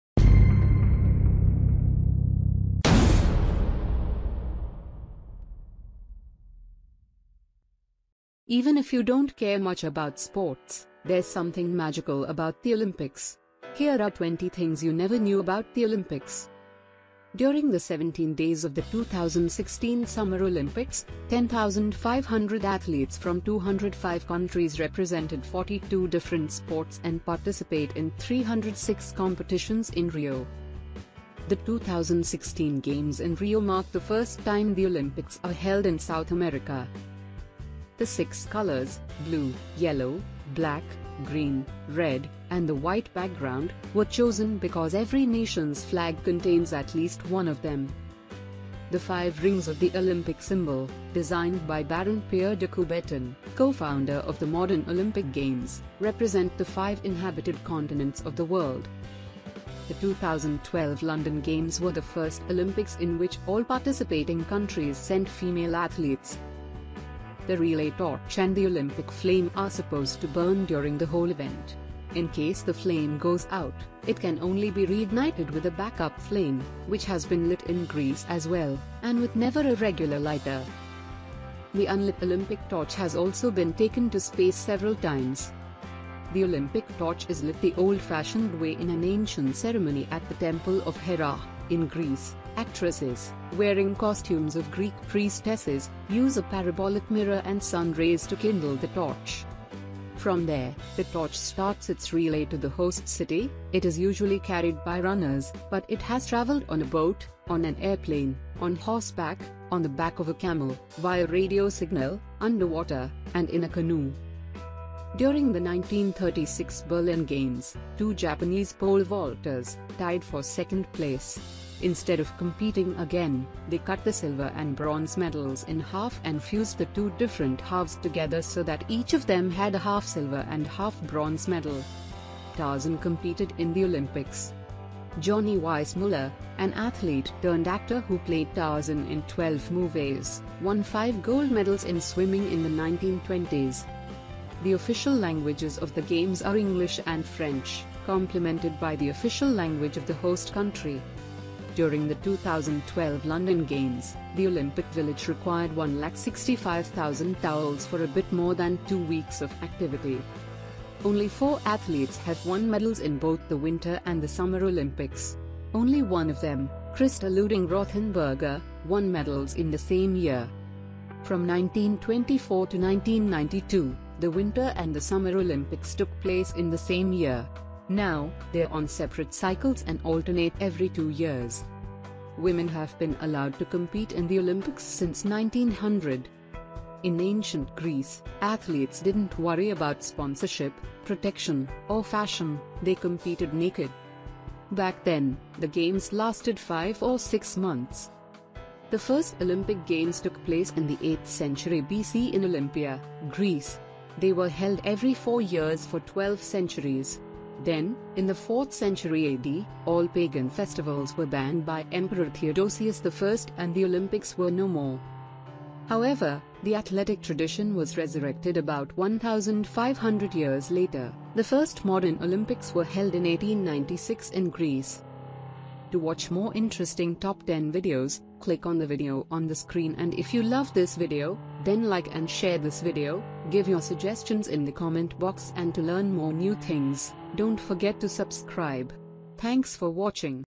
20 Things You Never Knew About The Olympics
Even if you don’t care much about sports, there’s something magical about the Olympics. Here are 20 Things You Never Knew About The Olympics.

1. However, the athletic tradition was resurrected about 1500 years later: The first modern Olympics were held in 1896 in Greece.
2. The first Olympic Games took place in the 8th century B.C. in Olympia, Greece. They were held every four years for 12 centuries. Then, in the 4th century A.D., all pagan festivals were banned by Emperor Theodosius I and the Olympics were no more.
3. Back then, the games lasted five or six months.
4. In ancient Greece, athletes didn’t worry about sponsorship, protection, or fashion – they competed naked.
5. Women have been allowed to compete in the Olympics since 1900.
6. From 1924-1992, the Winter and the Summer Olympics took place in the same year. Now, they’re on separate cycles and alternate every two years.
7. Only four athletes have won medals in both the Winter and the Summer Olympics. Only one of them, Christa Luding-Rothenburger, won medals in the same year.
8. During the 2012 London Games, the Olympic Village required 165,000 towels for a bit more than two weeks of activity.
9. The official languages of the games are English and French, complemented by the official language of the host country.
10. Tarzan competed in the Olympics:
Johnny Weissmuller, an athlete-turned-actor who played Tarzan in 12 movies, won five gold medals in swimming in the 1920s.
11. During the 1936 Berlin Games, two Japanese pole-vaulters tied for second place. Instead of competing again, they cut the silver and bronze medals in half and fused the two different halves together so that each of them had a half-silver and half-bronze medal.
12. From there, the torch starts its relay to the host city: It is usually carried by runners, but it has traveled on a boat, on an airplane, on horseback, on the back of a camel, via radio signal, underwater, and in a canoe.
13. The Olympic torch is lit the old-fashioned way in an ancient ceremony at the temple of Hera, in Greece: Actresses, wearing costumes of Greek priestesses, use a parabolic mirror and sun rays to kindle the torch.
14. The unlit Olympic torch has also been taken to space several times.
15. The relay torch and the Olympic flame are supposed to burn during the whole event. In case the flame goes out, it can only be reignited with a backup flame, which has been lit in Greece as well, and with never a regular lighter!
16. The 2012 London Games were the first Olympics in which all participating countries sent female athletes.
17. The five rings of the Olympic symbol – designed by Baron Pierre de Coubertin, co-founder of the modern Olympic Games – represent the five inhabited continents of the world.
18. The six colors – blue, yellow, black, green, red, and the white background – were chosen because every nation’s flag contains at least one of them.
19. The 2016 Games in Rio mark the first time the Olympics are held in South America.
20. During the 17 days of the 2016 Summer Olympics, 10,500 athletes from 205 countries represented 42 different sports and participate in 306 competitions in Rio.

Thanks for Your Subscription and support to Infinite Expertise.